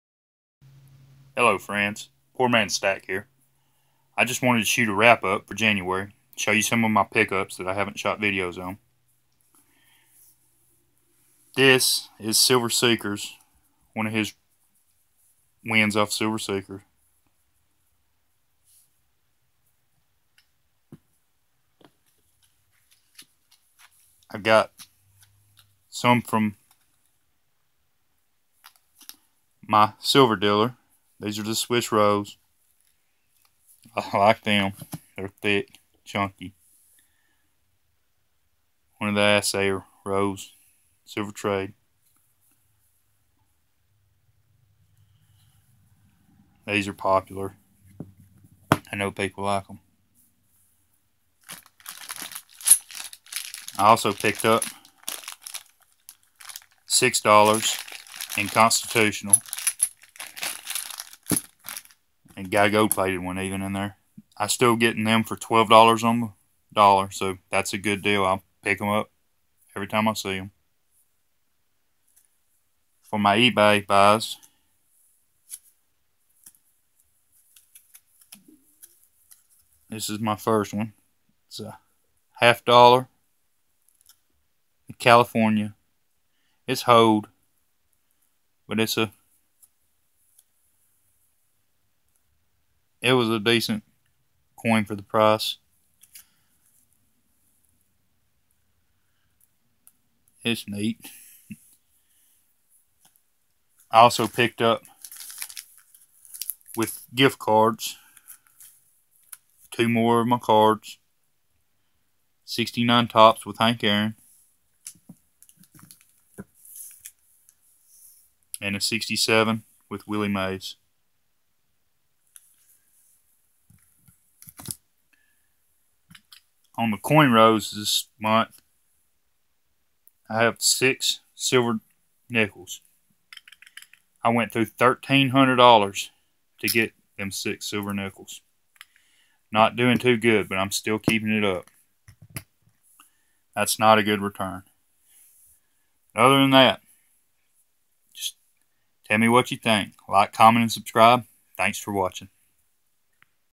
January pick-ups.
Slow month but some pick-ups from January. The gold 1/2 is 1872 and fumble mouth the Hank is a 1968 sorry. I can't find any silver in the bank rolls only find silver in customer wrapped rolls.